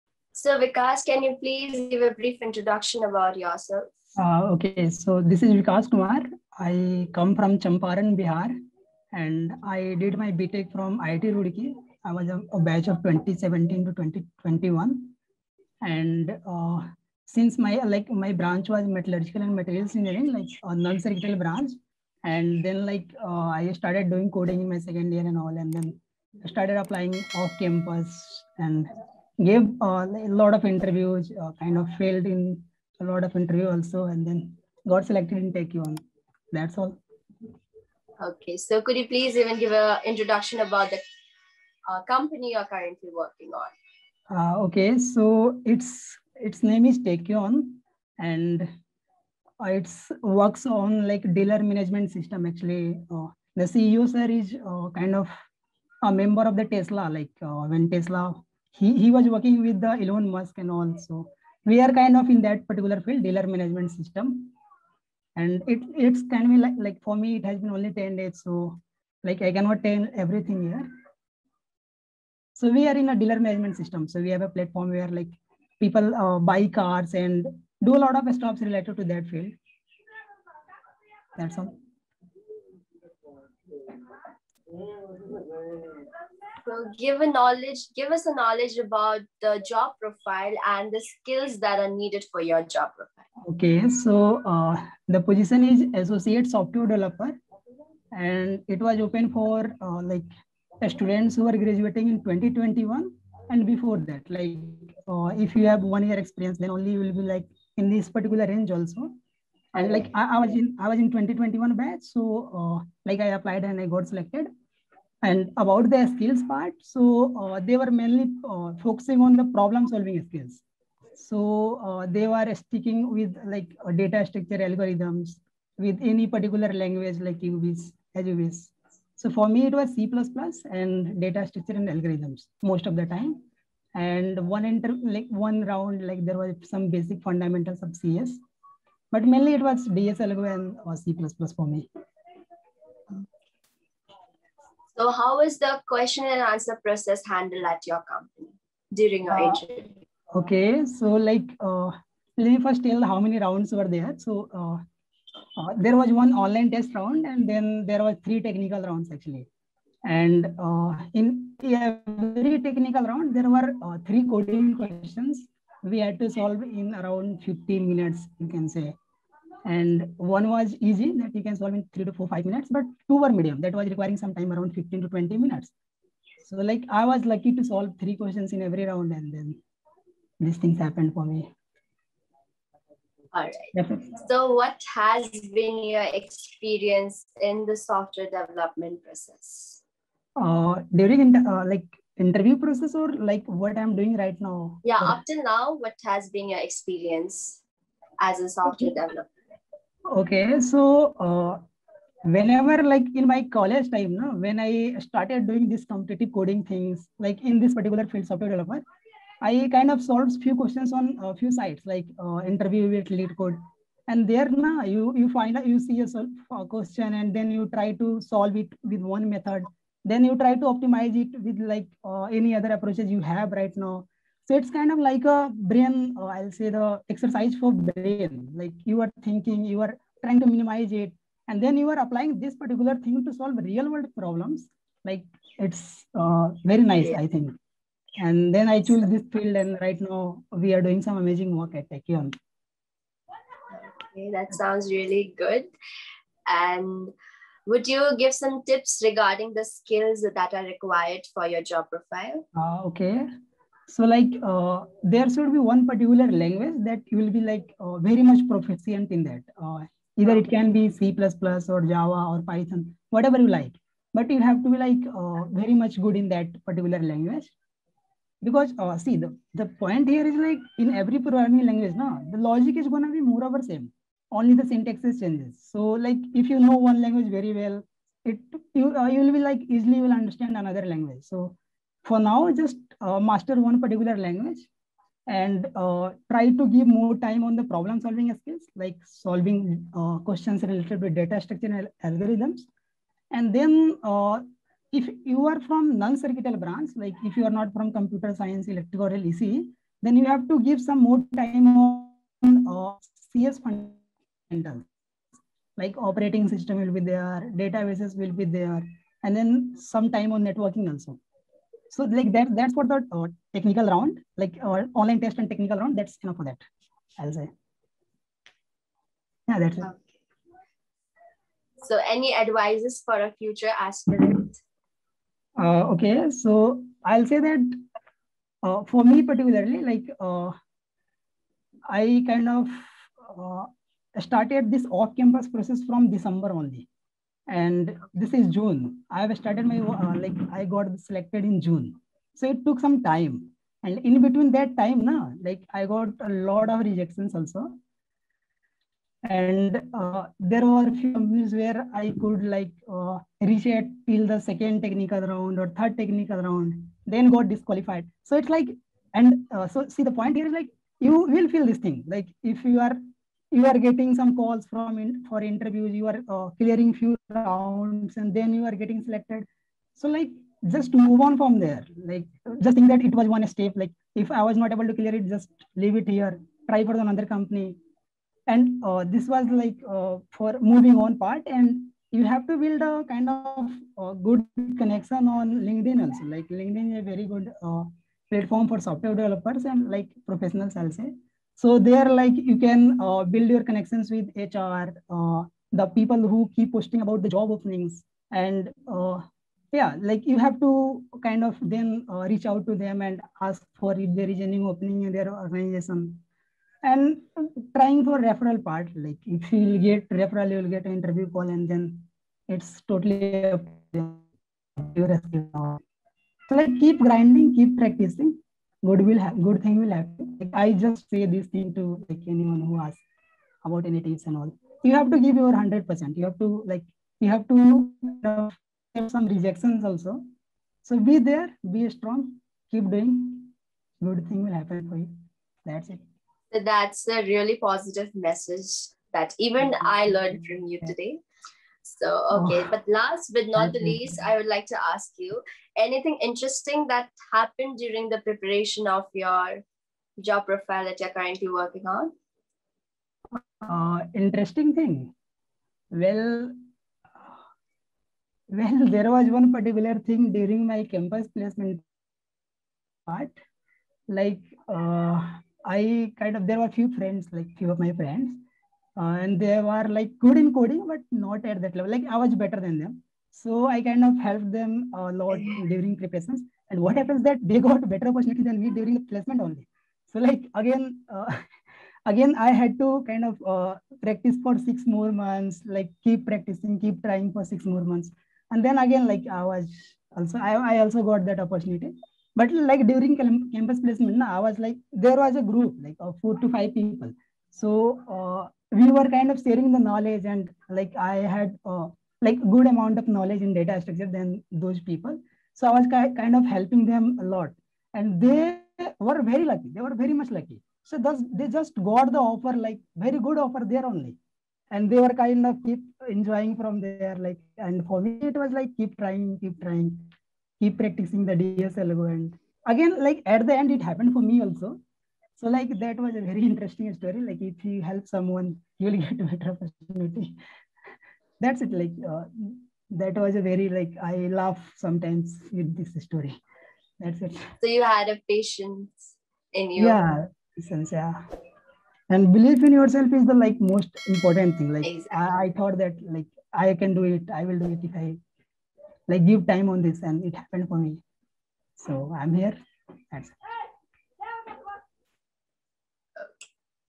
Software Developer interview | Tekion corp
Today we have with us a passionate software developer, Vikash Kumar .
A simple living man with big dreams and aspirations . His knee interest is  problem-solving with different methodologies and technologies such as C++, Golang, Data Structure and Algorithms, MongoDB, SQL, Postman, Low-level design etc.

He is  always eager to learn something new and implement them in the best possible way.

He has shared his true based experience and a life long learning . He works as an associate software developer at Tekion corp

He has mentioned few advantages and skills and resources that are required to get through the interview he has also mentioned about the interview questions too .